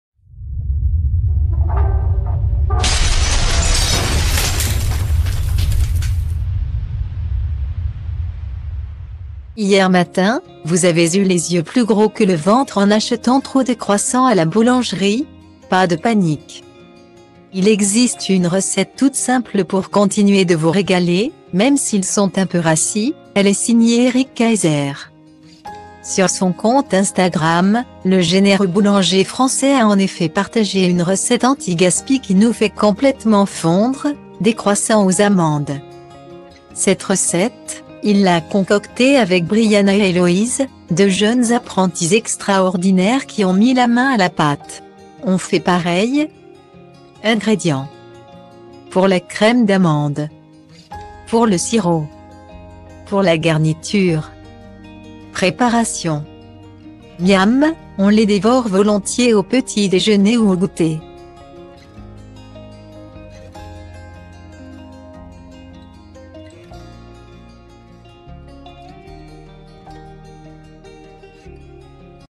Eric Kayser : sa recette de croissants amandes, à faire avec ses enfants
Hier matin, vous avez eu les yeux plus gros que le ventre en achetant trop de croissants à la boulangerie ? Pas de panique,il existe une recette toute simple pour continuer de vous régaler, même s’ils sont un peu rassis ; elle est signée Eric Kayser.Sur son compte Instagram, le généreux boulanger français a en effet partagé une recette antigaspi qui nous fait complètement fondre : des croissants aux amandes.